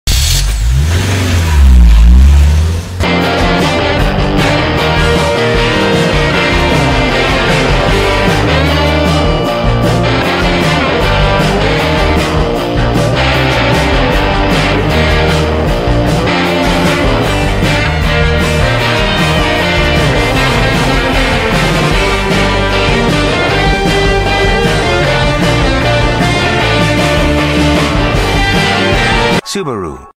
Subaru WRX "Ignite" Digital Ad Q4 2022-2023 30s (Philippines, With "Made in Japan" Version)
Unleash your driving passion with the Subaru WRX. Ignite your senses and find out more about the Subaru WRX and WRX Wagon today.

DRPS One 7976: Subaru WRX "Ignite" Digital Ad Q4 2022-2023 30s (Philippines, With "Made in Japan" Version)